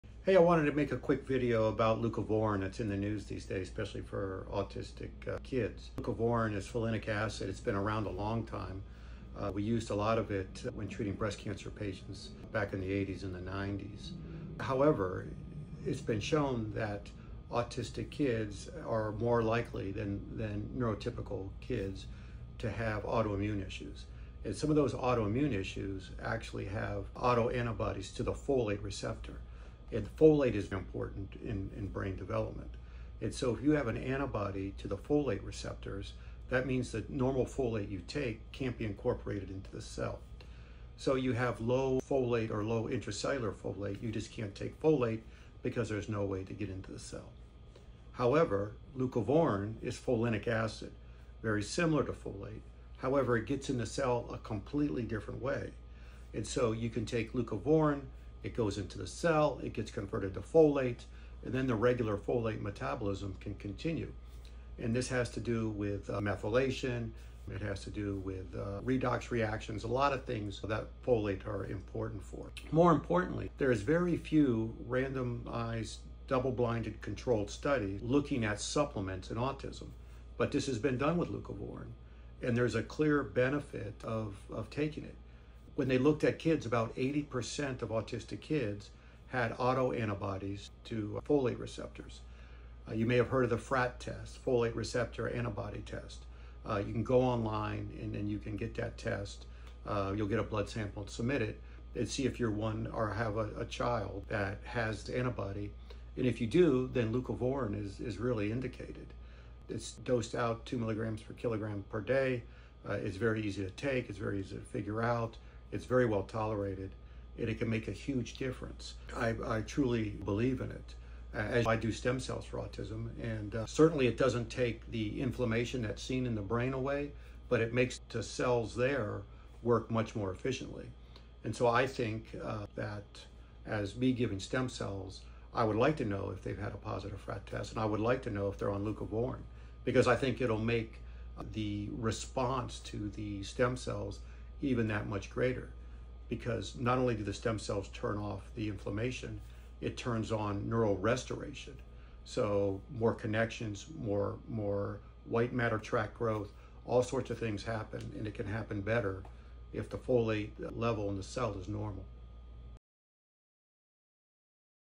Leucovorin for children ASD
Dr. Weiss breaks down how the prescription medication Leucovorin may be beneficial to children with autism. Listen to hime explain the science behind it why it works!